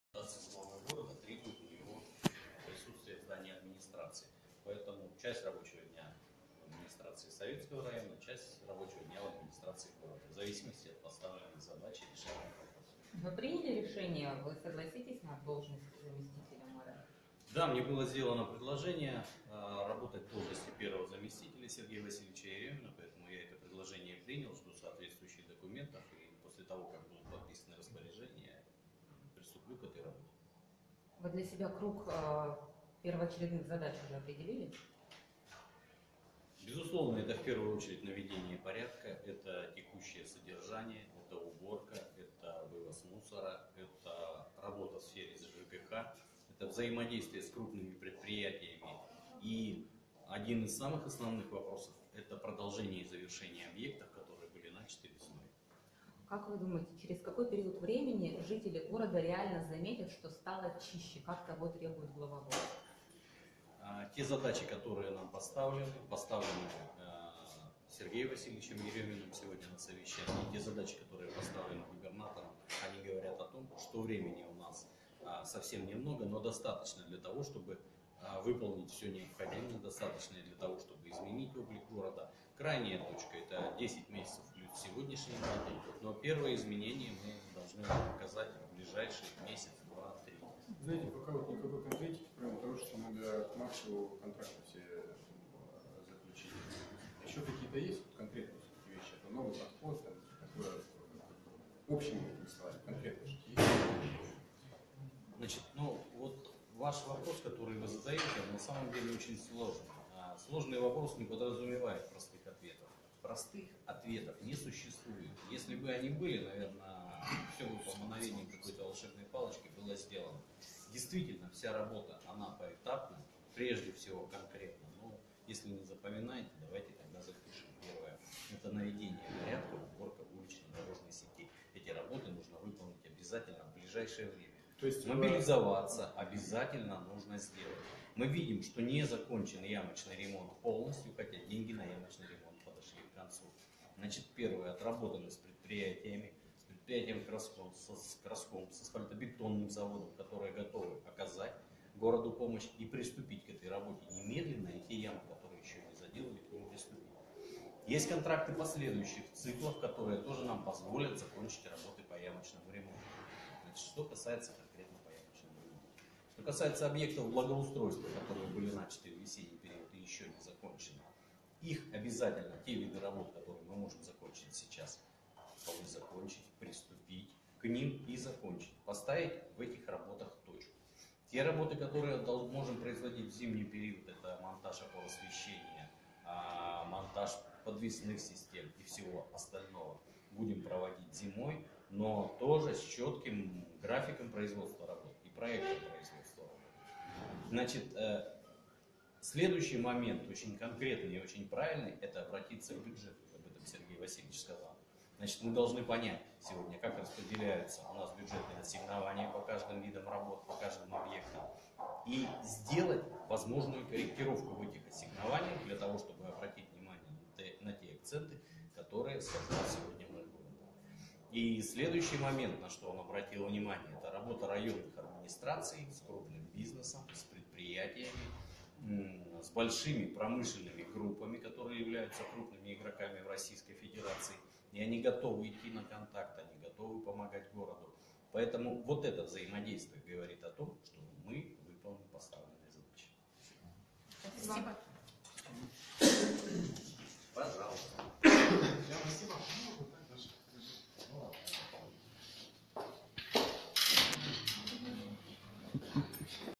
Владислав Логинов о предложении стать первым замом мэра Красноярска
Видео: Александр Черных